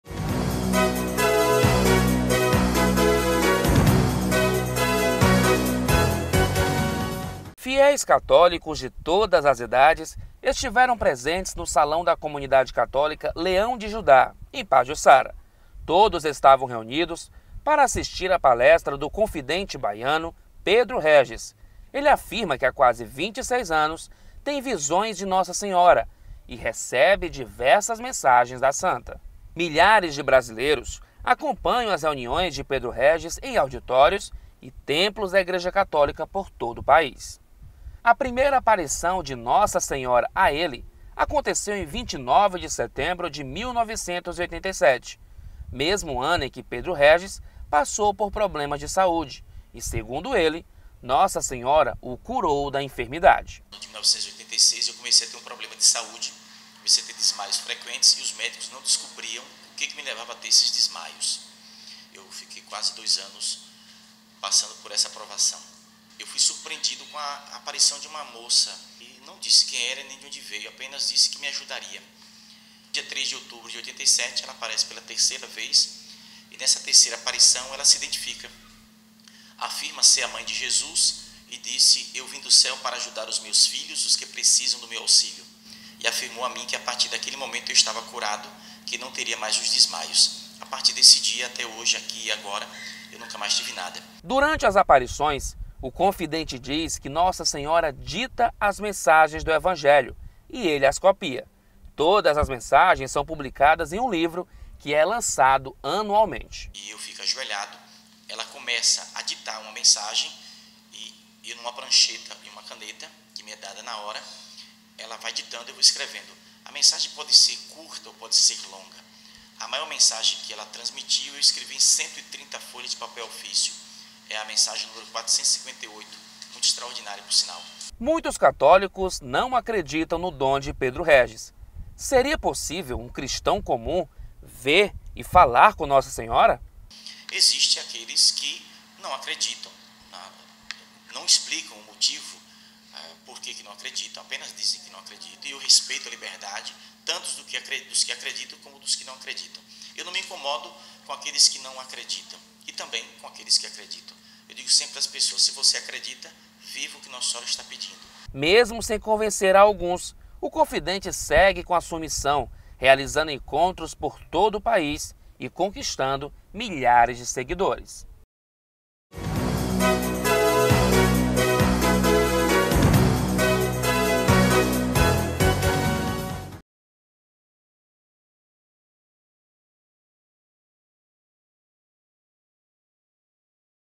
Homem que diz ver Nossa Senhora realiza palestra em Maracanaú
O confidente Pedro Regis, natural de Anguera - BA, afirma ver e falar com Nossa Senhora, ele esteve na comunidade católica Leão de Judá no bairro da Pajuçara e realizou palestra para quase 500 fiéis. Confira na reportagem.